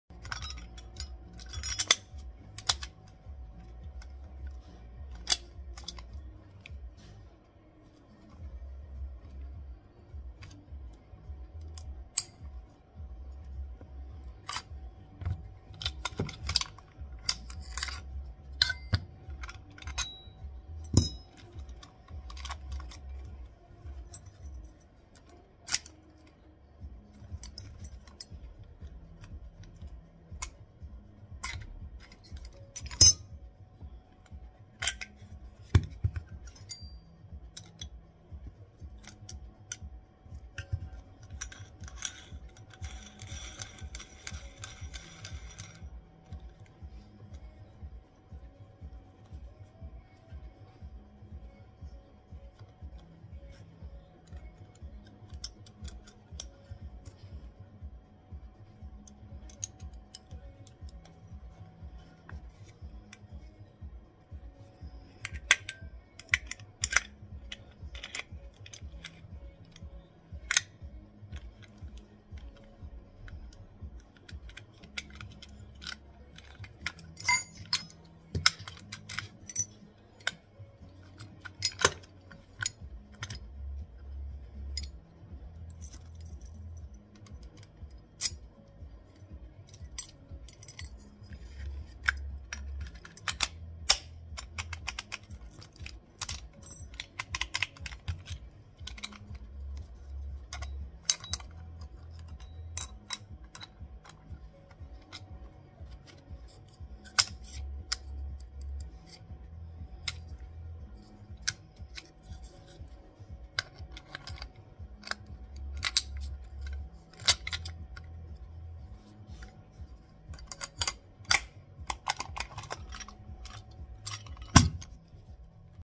Disassemble and reassemble Brava Metro lever set
This is a Brava Metro EU storeroom set, but the general technique suits a range of locksets - particularly turning the key 90° clockwise and depressing the tab within.